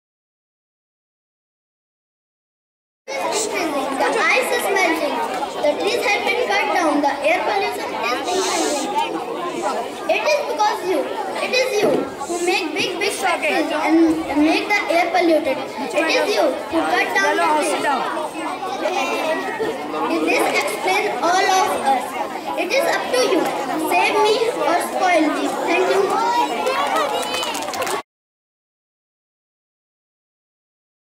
SAVE EARTH (NO 5 KENDRIYA VIDYALAYA STUDENT, AFS DARJIPURA ) AT CMP, HARNI ON(19-11- 2016)
SAVE EARTH  (NO 5 KENDRIYA VIDYALAYA STUDENT, AFS DARJIPURA ) AT CMP, HARNI, VADODARA, GUJARAT, INDIA ON(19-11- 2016), VIDEOGRAPHY AND EDITING Mr. JAYANTA KUMAR MANDAL